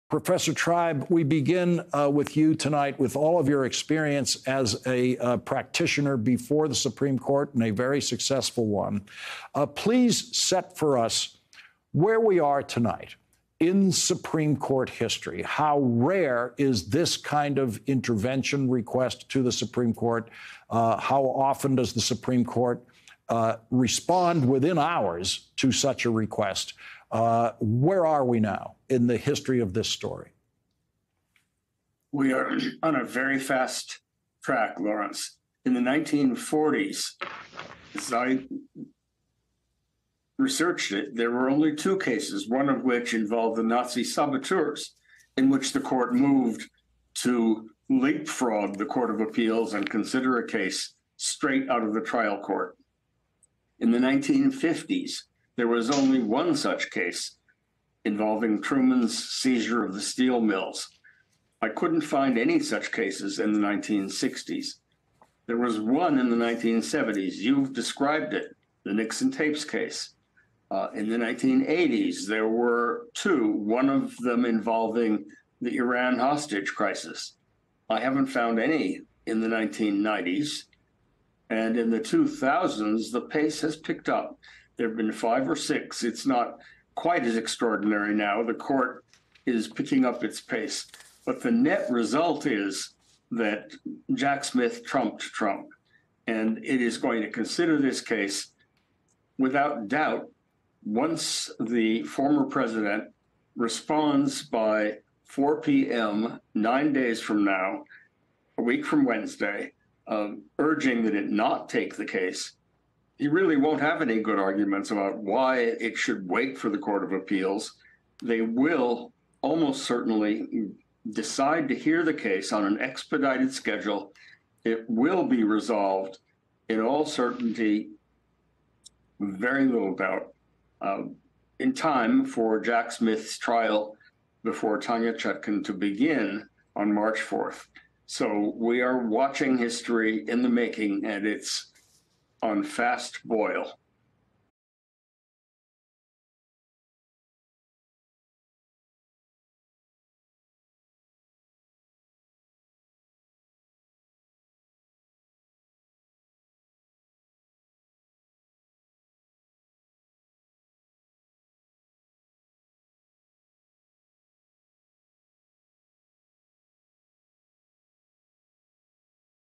'History on a fast boil': Tribe on Jack Smith taking Trump case to SCOTUS
Harvard Constitutional Law Professor Laurence Tribe joins MSNBC's Lawrence O'Donnell to discuss Special Counsel Jack Smith seeking the Supreme Court to rule on whether Trump is immune from prosecution in Smith's federal elections case against Trump.